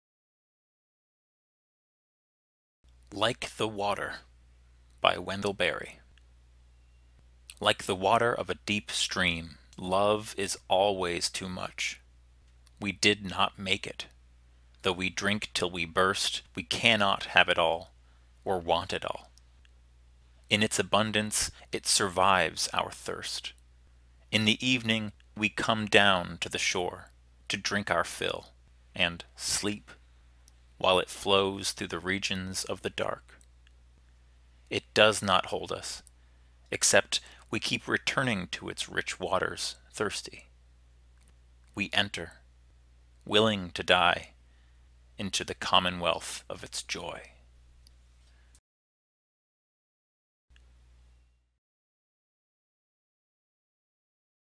Like the Water, by Wendell Berry
Like The Water - Poem by Wendell Berry

Like the water
of a deep stream,
love is always too much.
We did not make it.
Though we drink till we burst,
we cannot have it all,
or want it all.
In its abundance
it survives our thirst.

In the evening we come down to the shore
to drink our fill,
and sleep,
while it flows
through the regions of the dark.
It does not hold us,
except we keep returning to its rich waters
thirsty.

We enter,
willing to die,
into the commonwealth of its joy.